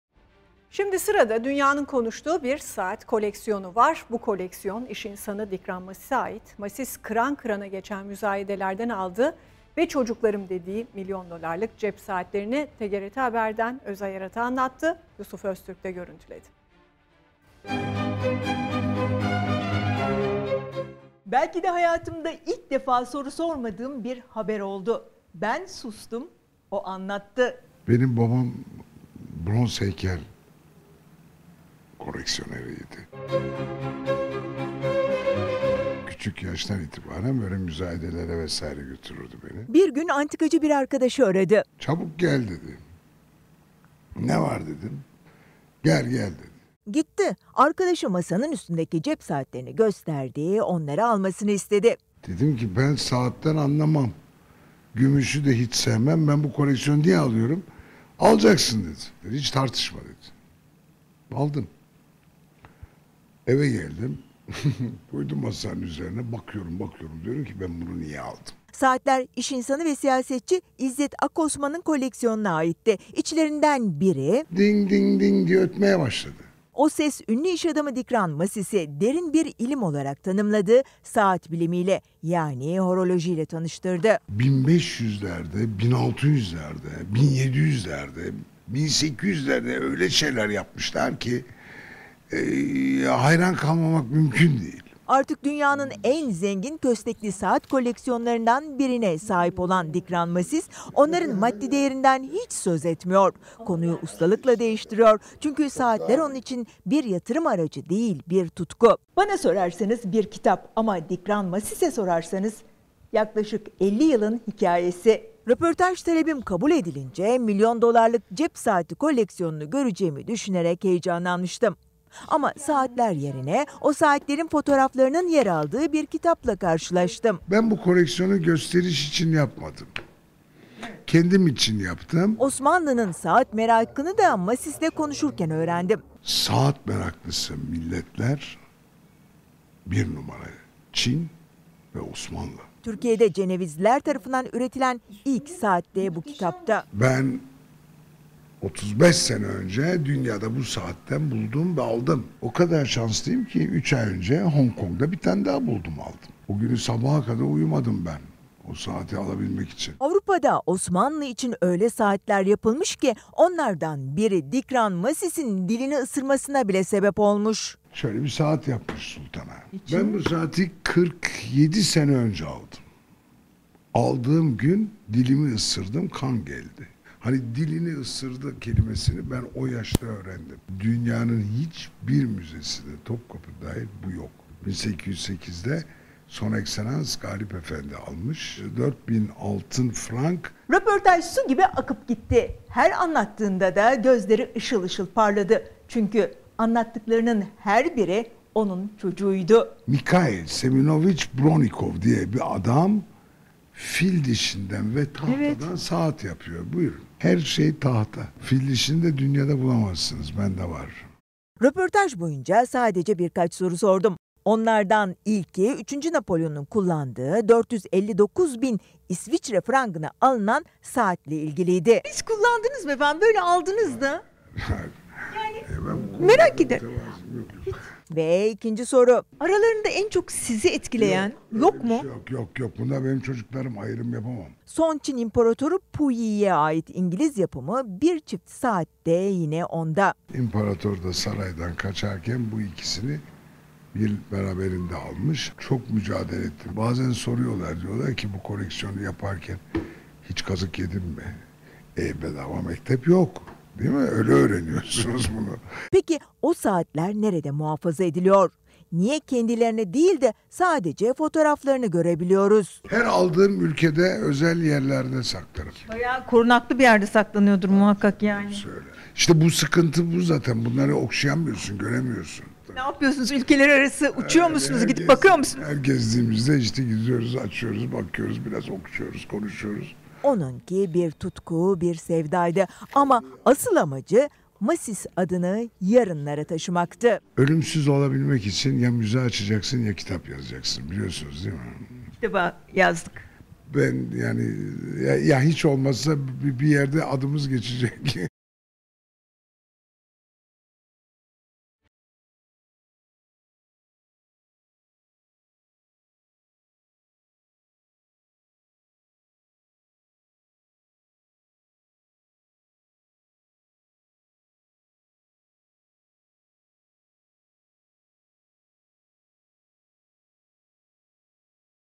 Paha Biçilemeyen Cep Saatlerinin Efendisi! - TGRT Özel Haber
Dünyanın konuştuğu bir saat koleksiyonu var. Bu koleksiyon iş insanı Dikran Masis'e ait. Masis, kıran kıran geçen müzayedelerden aldığı ve 'çocuklarım' dediği milyon dolarlık cep saatlerini TGRT Haber'den Özay Erad'a anlattı.